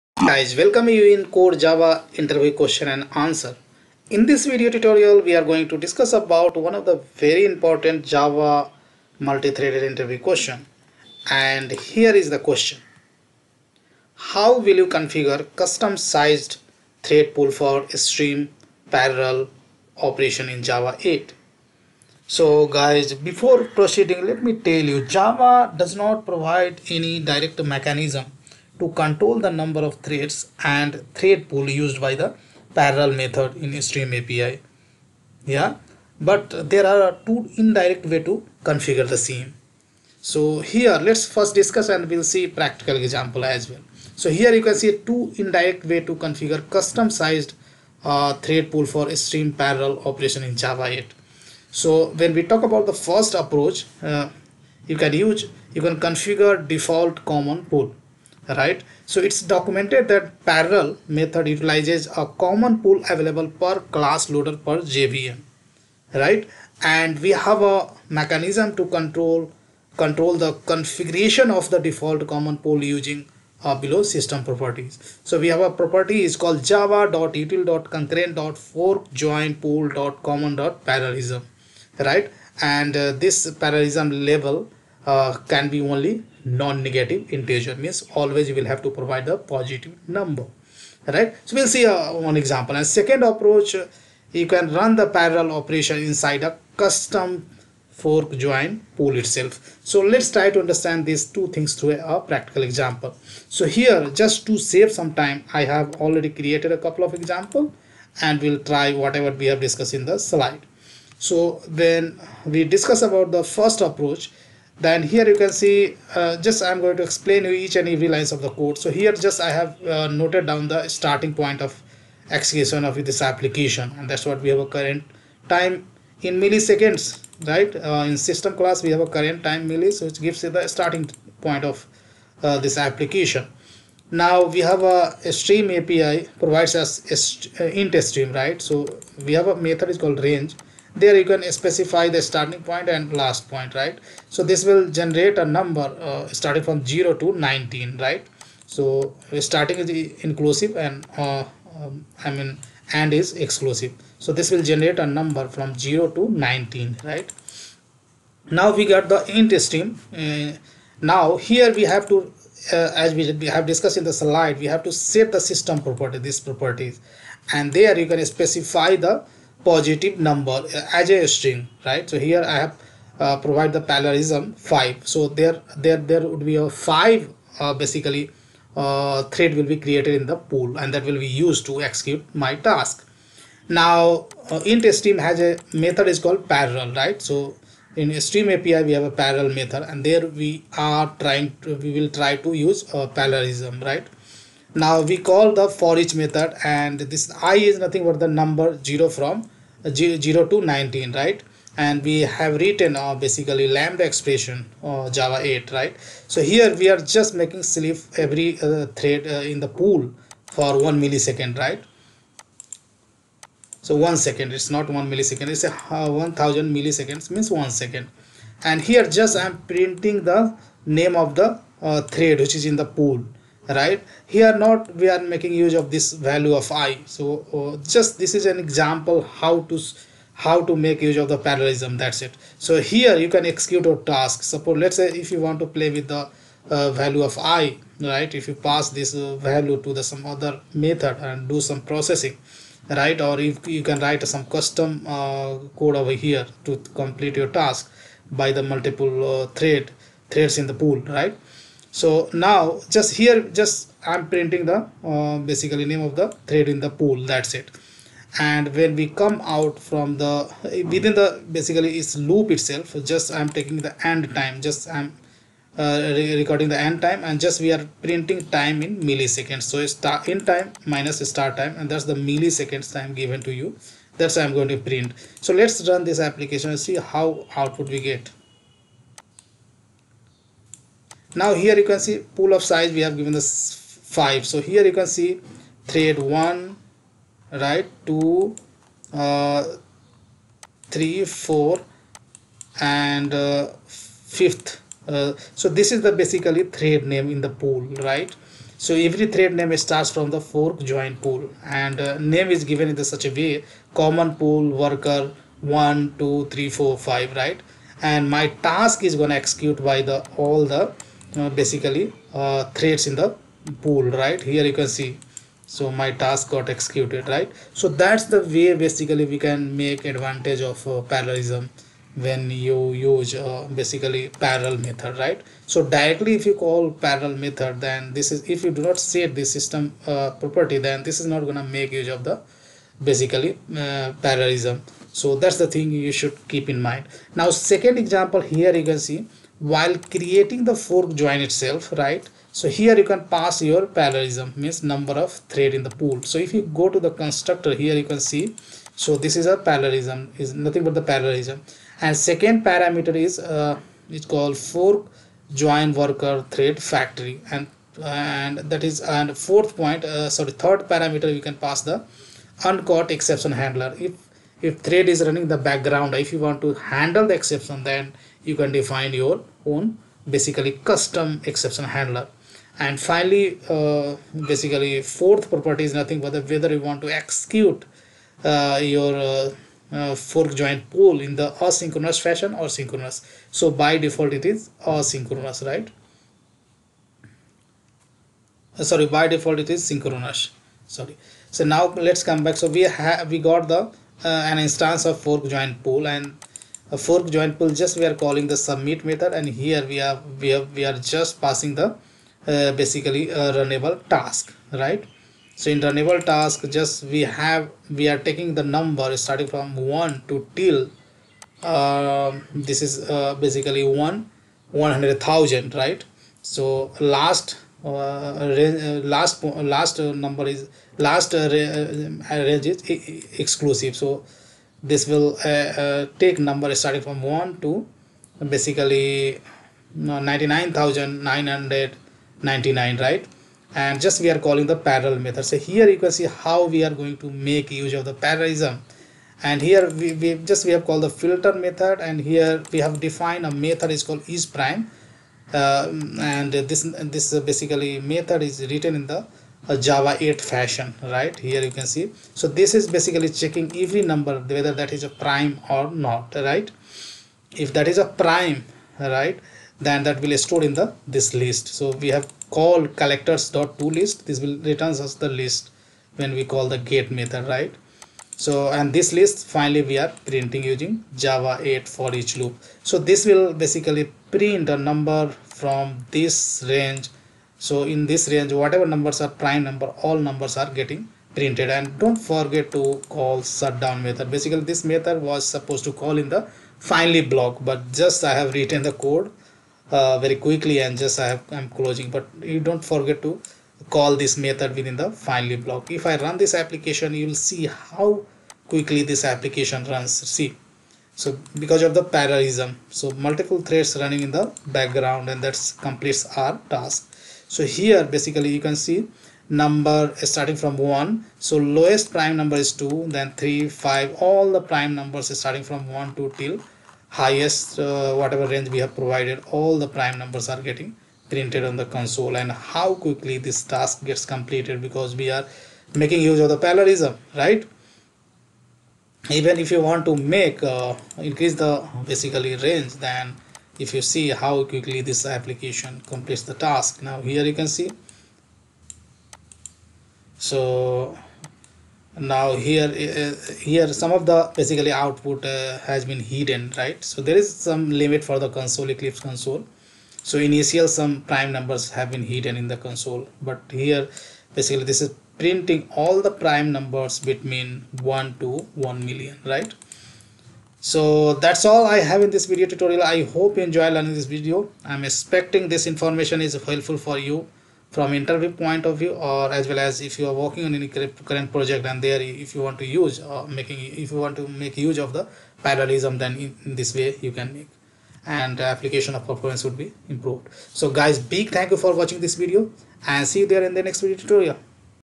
How will you configure custom sized ThreadPool for stream parallel operation in Java 8 ?
About this Video:
Hello Friends, In this video we will talk and learn one of the very important interview question How will you configure custom sized ThreadPool for stream parallel operation in Java 8 ?